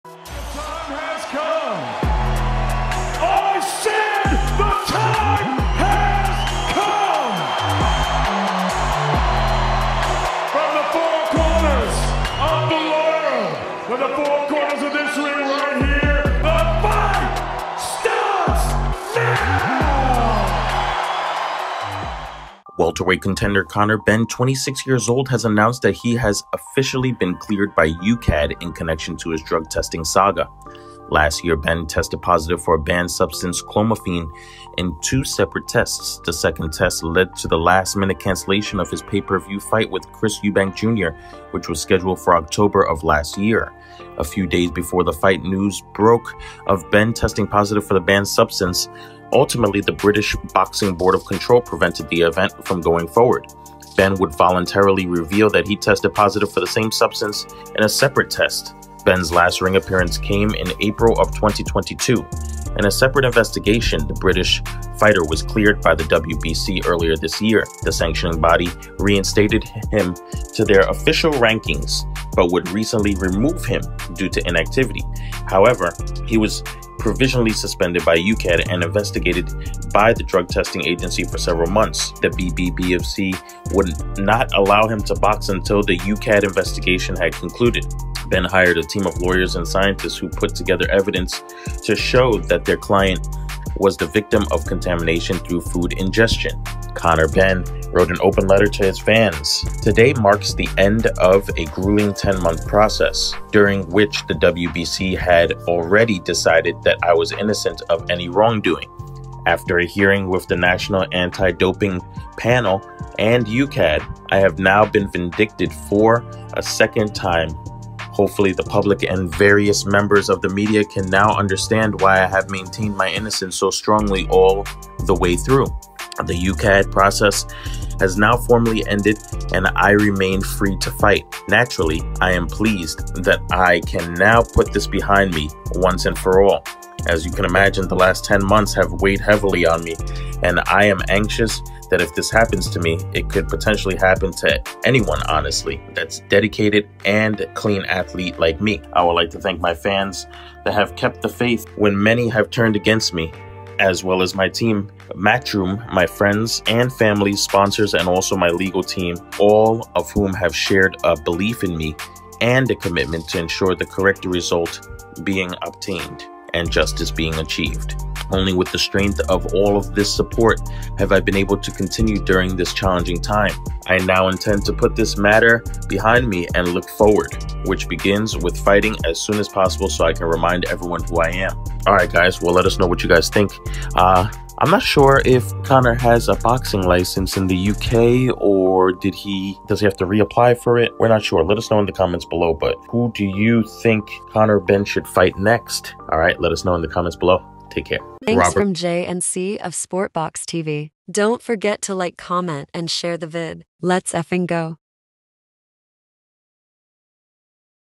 Conor Benn Open Letter Stating He Has Been Cleared To Fight By UKAD & Will Be Fighting Soon
In this boxing news segment Conor BENN Officially Cleared, Cortes Stops Martinez, Itauma Says Dubois Harder Spar Than AJ…

Welterweight contender Conor Benn, 26-years-old, has announced that he was officially cleared by UKAD [UK Anti-Doping] in connection to his drug testing saga.

Last year, Benn tested positive for banned substance clomifene in two separate tests.

The second test led to the last-minute cancelation of his pay-per-view fight with Chris Eubank Jr. - which was scheduled for October of last year.

**This video was edited by the owners of this channel & ANY & ALL “Fair-Use” Content have been properly REMIXED To Original Content*